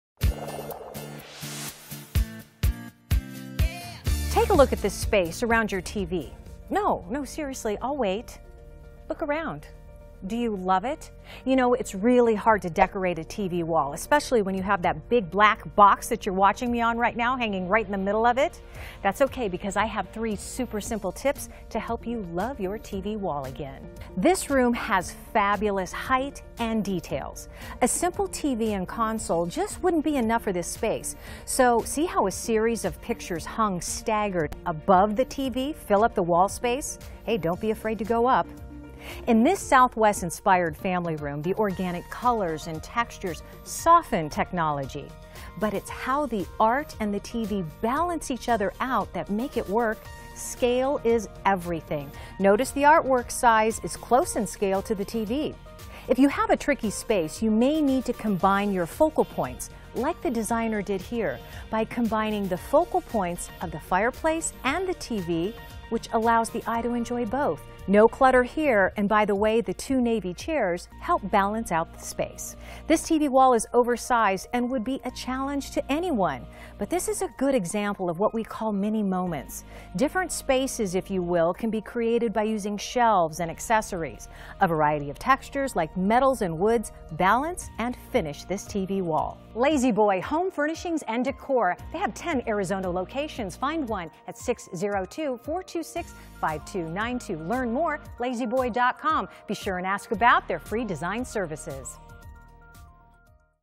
How to decorate your TV Wall.
With wall mounted TV's instead of entertainment centers, the TV wall can be tricky to decorate. Here are a few simple tips to help.